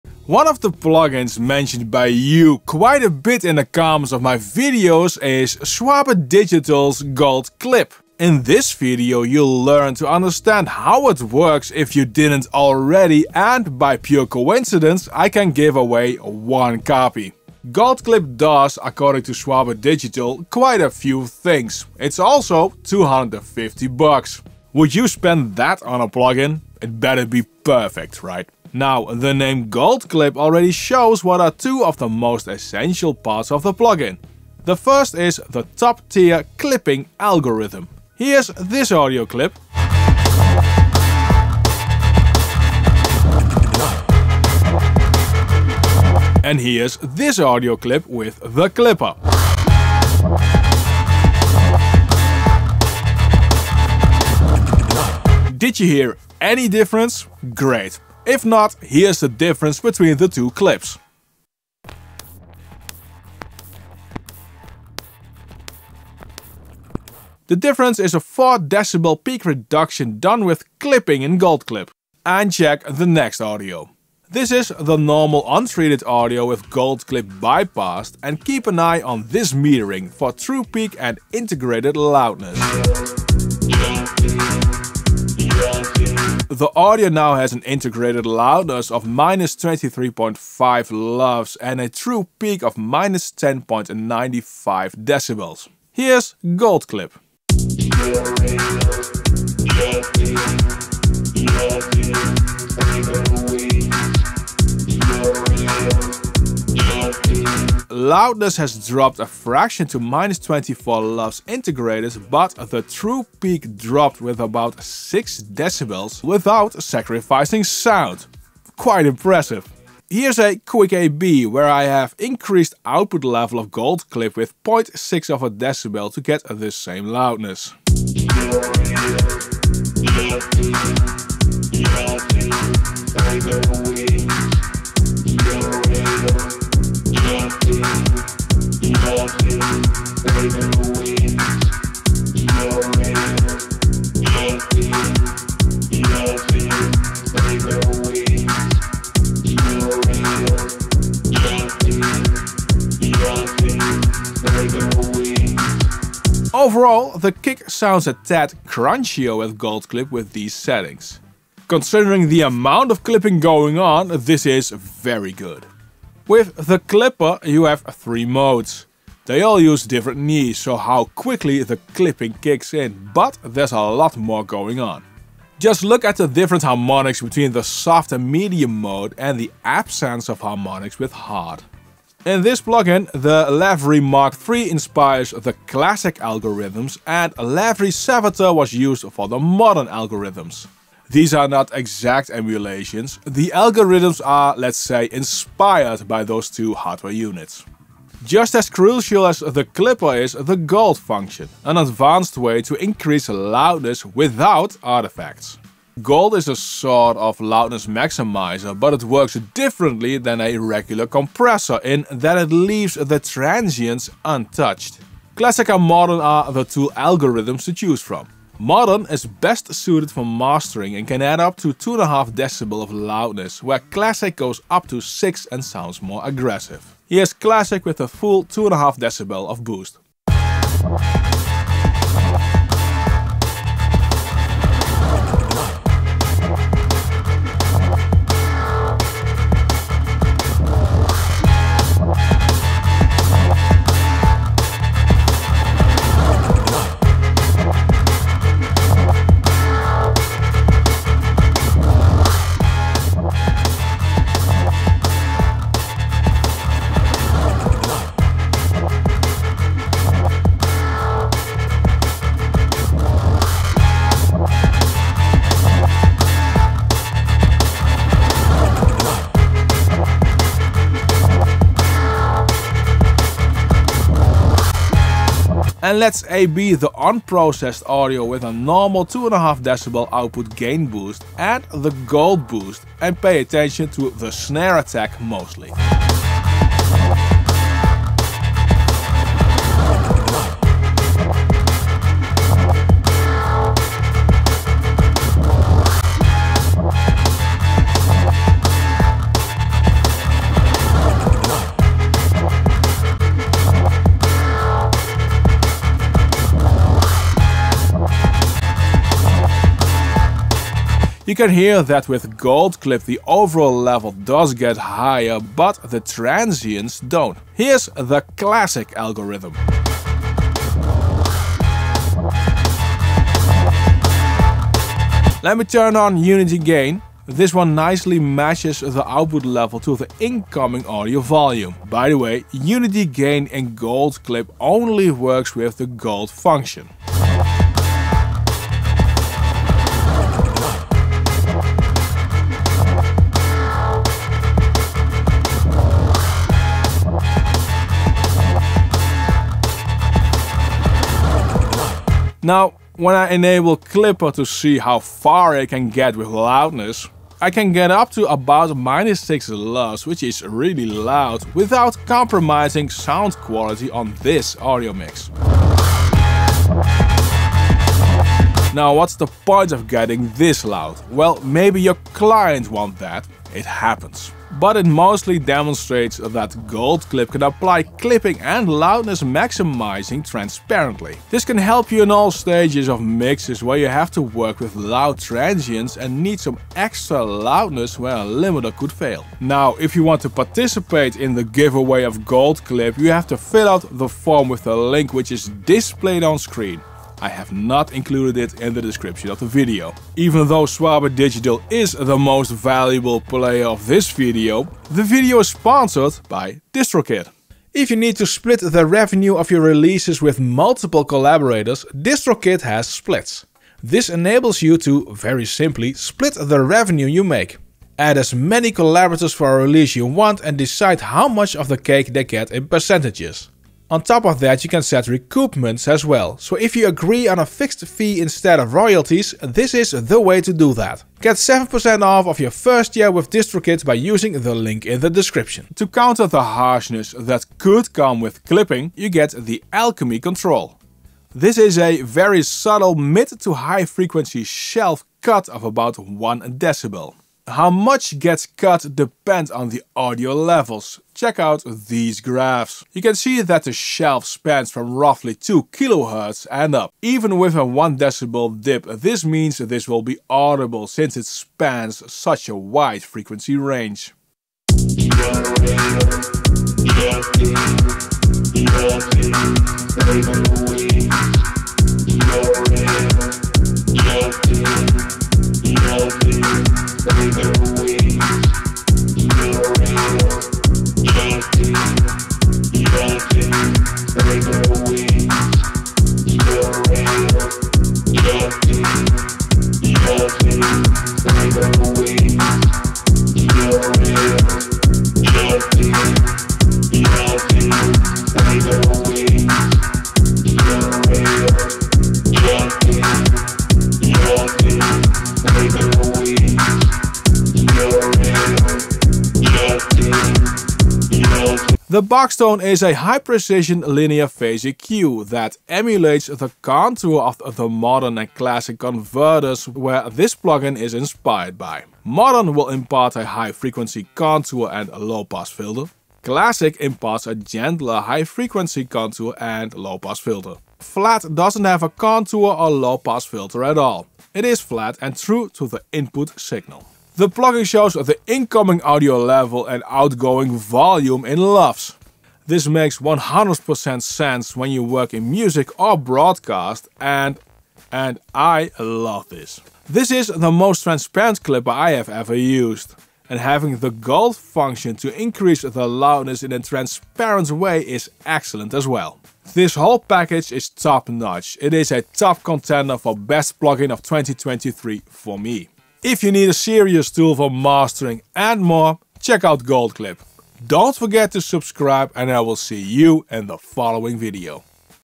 The BEST loudness plugin? Schwabe Digital GoldCLIP
Schwabe Digital's Gold Clip may be the most transparant Clipper and Loudness Maximiser.

Plugin Deals – updated 10 July 2024
Free Plugin with every Plugin Boutique paid purchase:
Audiothing Frostbite 2 or Newfangled Audio EQuivocate

00:00 What is Schwabe Digital's GoldClip
00:35 Top tier Clipper
03:18 The Gold Loudness maximizer
07:44 Alchemy Clip Rolloff
08:31 Box Tone
08:56 Lufs Display